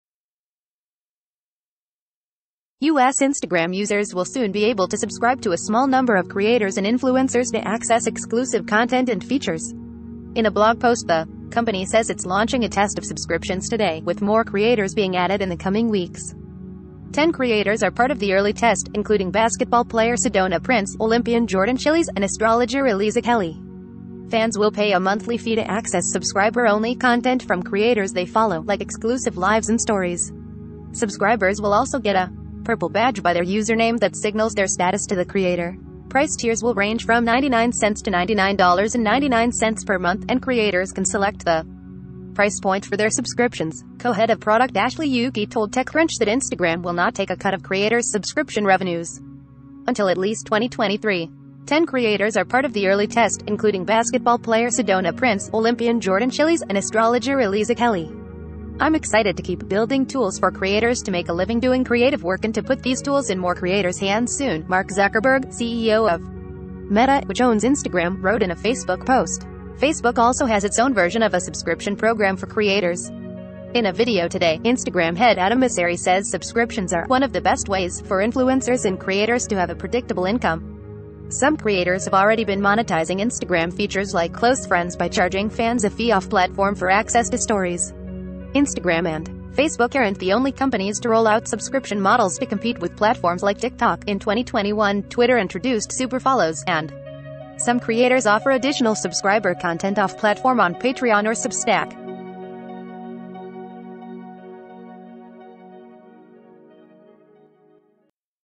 Instagram is testing paid subscriptions with a small group of creators - The Verge
In a blog post, thecompanysays it’s launching a test of subscriptions today, with more creators being added in the coming weeks.10 CREATORS ARE PART OF THE EARLY credit